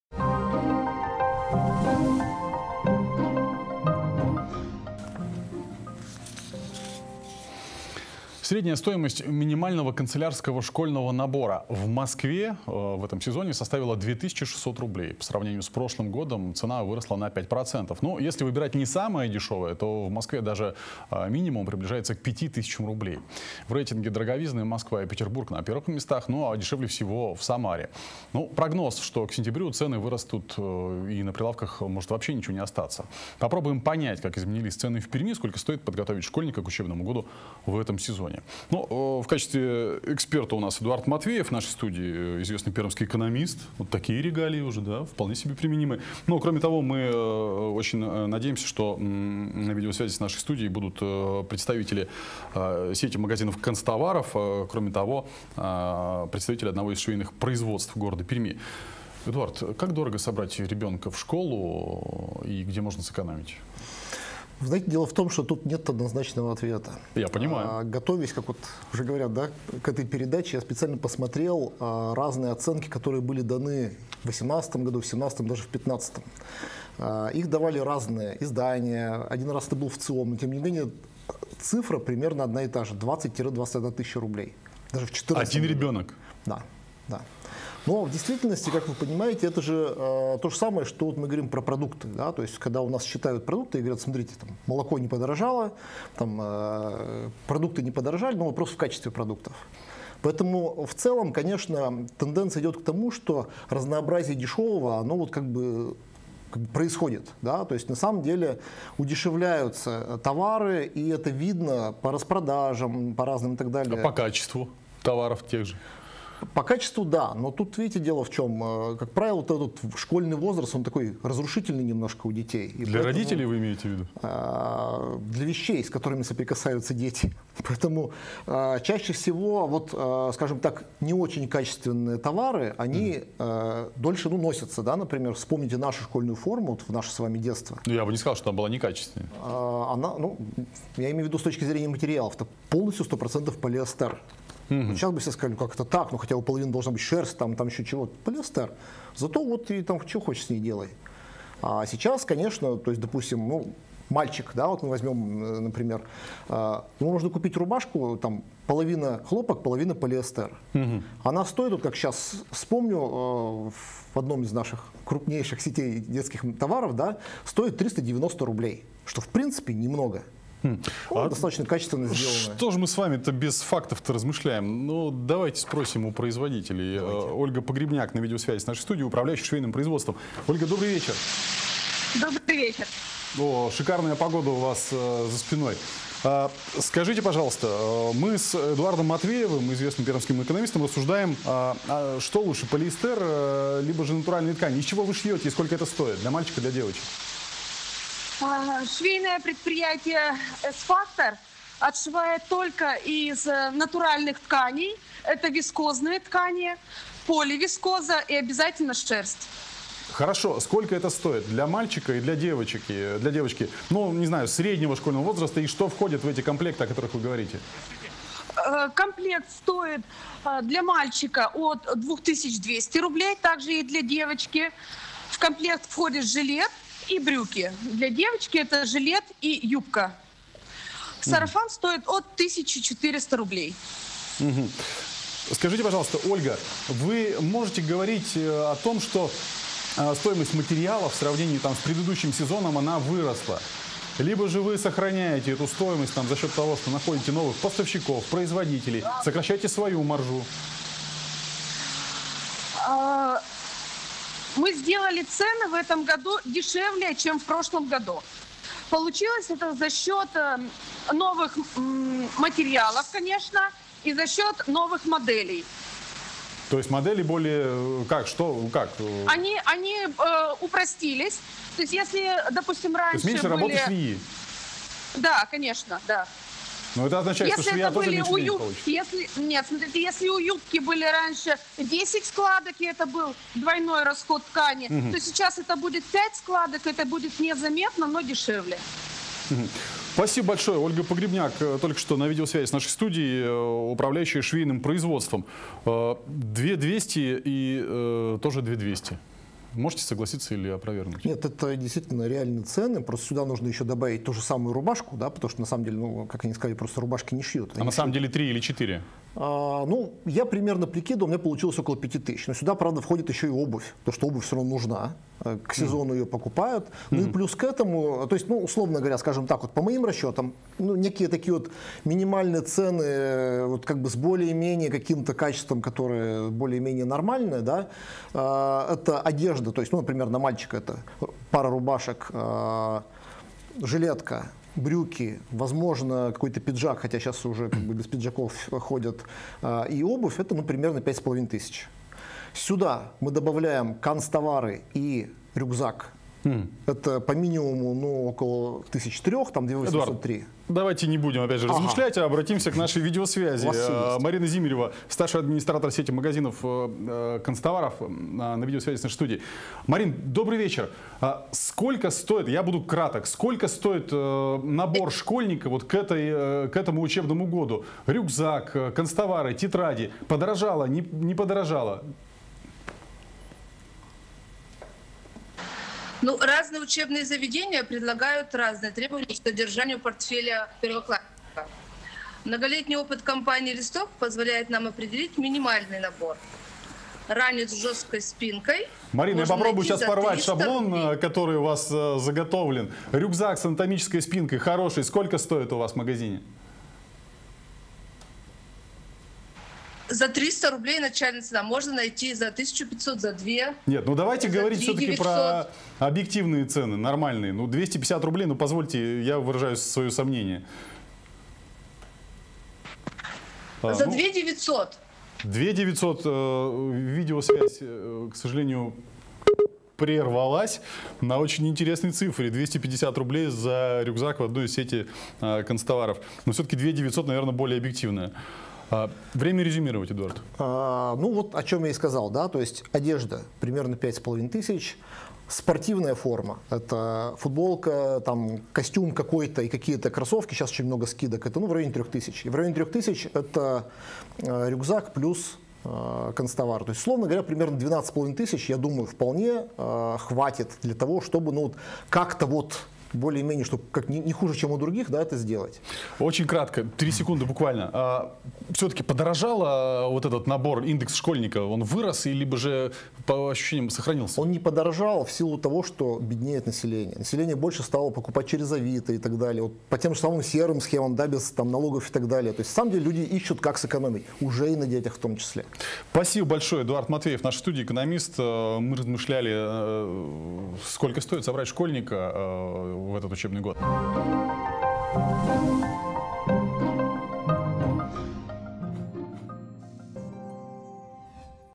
РБК-Пермь Итоги 30.07.19 Индекс школьника.
Как изменились цены на одежду и канцтовары?

Эксперты:
Эдуард Матвеев, экономист.
Ольга Погребняк, управляющая швейным производством.
Марина Зимирева, старший администратор сети магазинов канцтоваров.

Ведущий: Антон Шибанов.